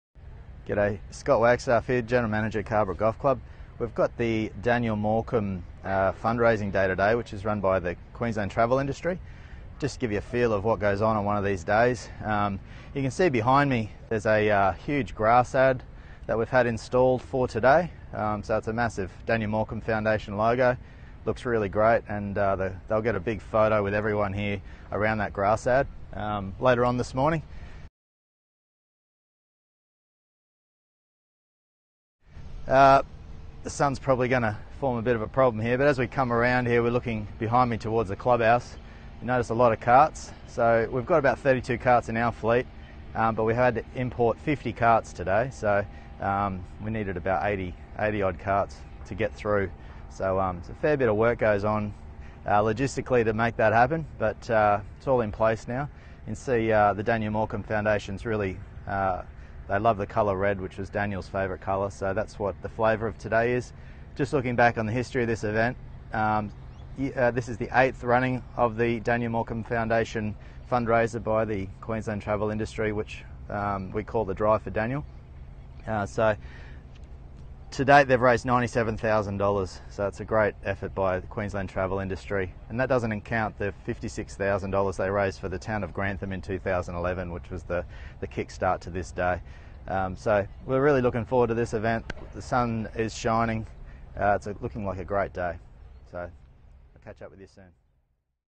Drive for Daniel Morcombe at Carbrook
Our GM Scott Wagstaff takes you through what's going on at the club with the Drive for Daniel Morcombe...